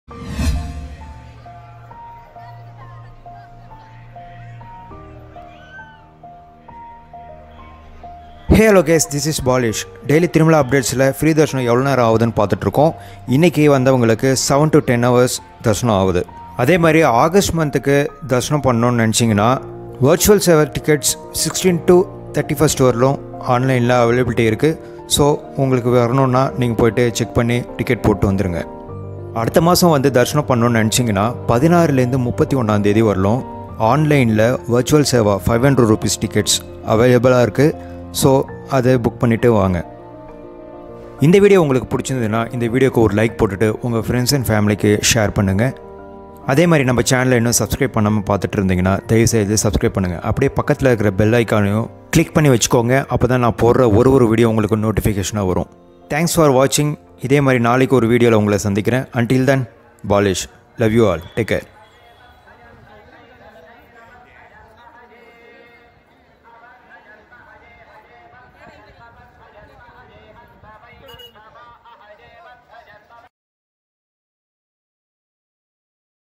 Tirumala Tirupathi | Daily Updates for free Darshan | Next August 500rs Darshan tickets available
Tirumala Tirupati Daily Updates for free Darshan and special Darshan tickets and more info.